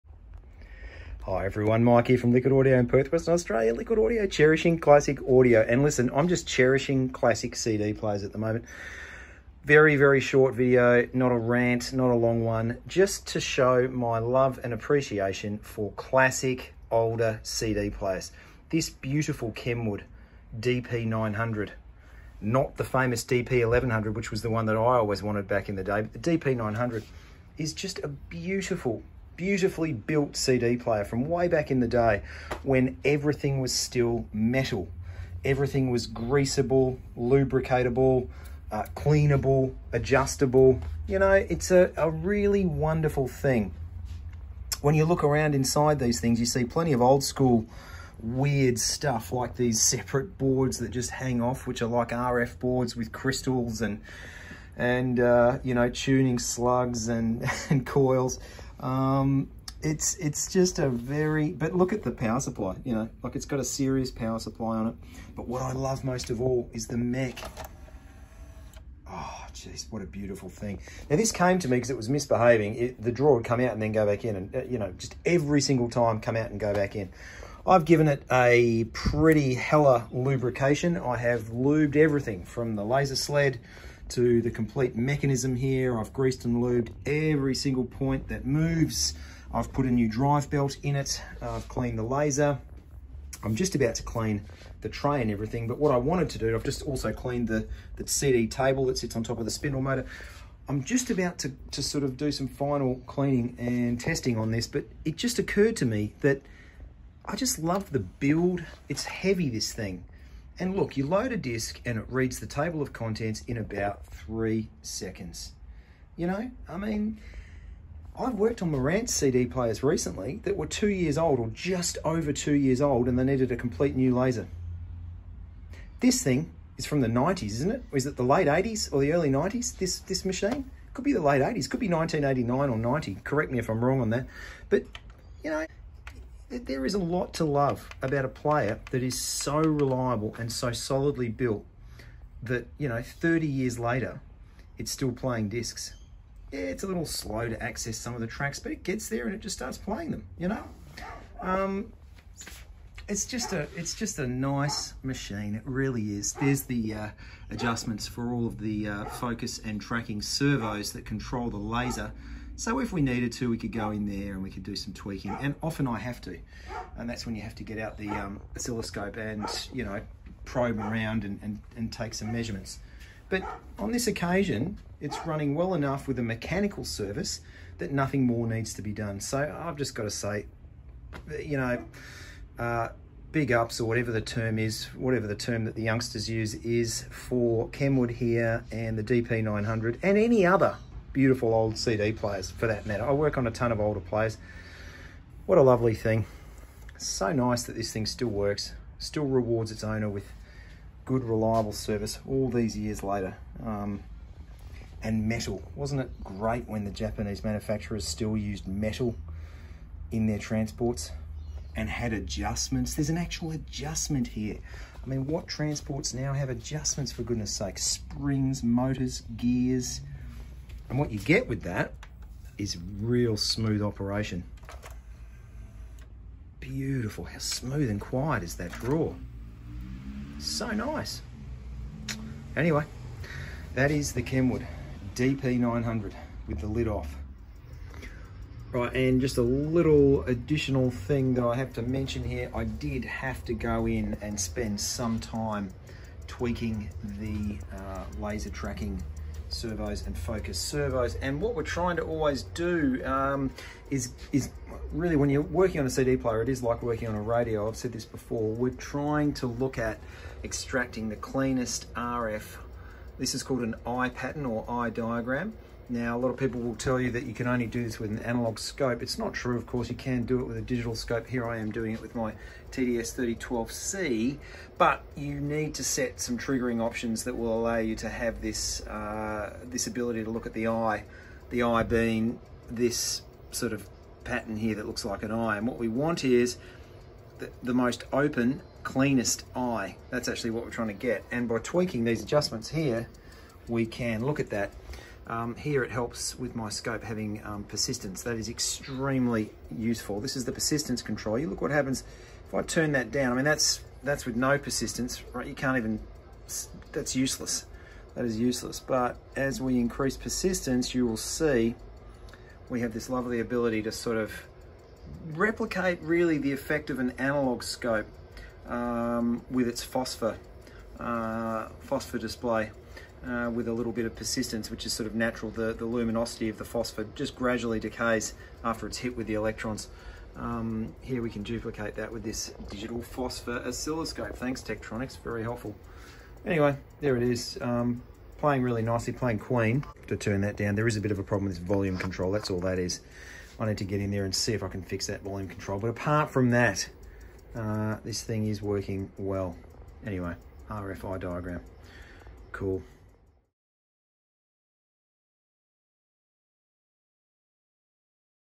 Lovely Kenwood DP-900 CD Player Repair & Service, plus RF Eye Diagram Discussion!
Join me this time for a short one where I rave on about another lovely older CD player. I love older CD players like this Kenwood DP-900. I repaired and serviced this one and as usual, forgot to say that the tray limit switch was causing the tray insanity problem here.

Just when I thought all was well, I had to spend a good while optimising the RF output from the laser. We look at the eye diagram on a Tektronix oscilloscope.

As always, please excuse the one-take, minimal editing, 'leave the goofs in' approach, it's the only way I can actually get videos out at the moment due to workload!

Liquid Audio is a specialist hi-fi equipment repairer based in Perth, Western Australia.